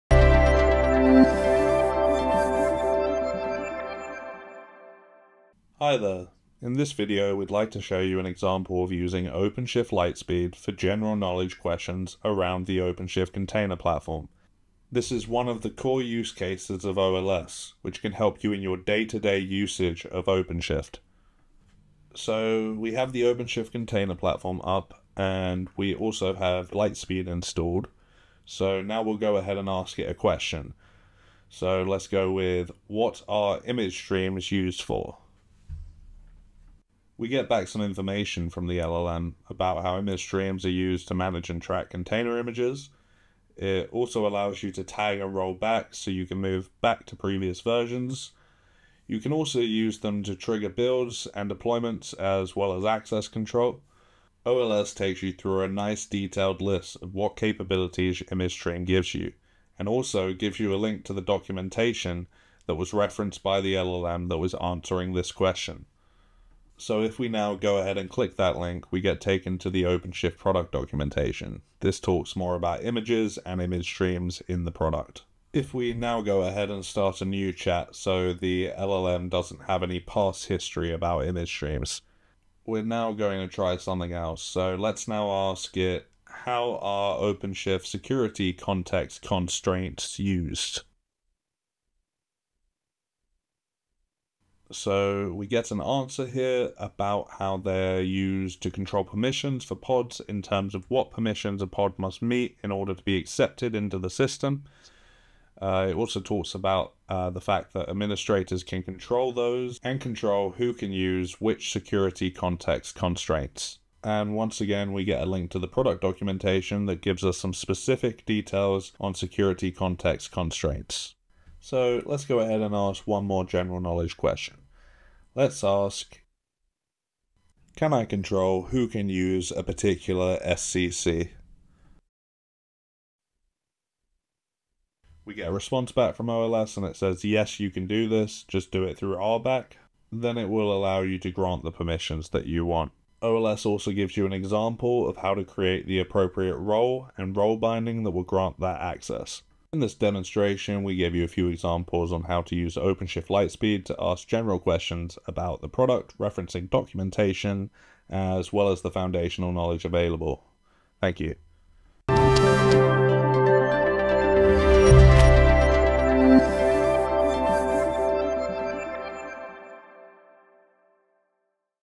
Ramp up your Red Hat OpenShift knowledge with OpenShift Lightspeed
Red Hat OpenShift Lightspeed is a genAI-based virtual assistant integrated directly into your Red Hat OpenShift web console. You can chat with it in natural English language and learn more about how to be more productive and efficient with Red Hat OpenShift.

Sign up for the Red Hat OpenShift Lightspeed Alpha program to help us improve the product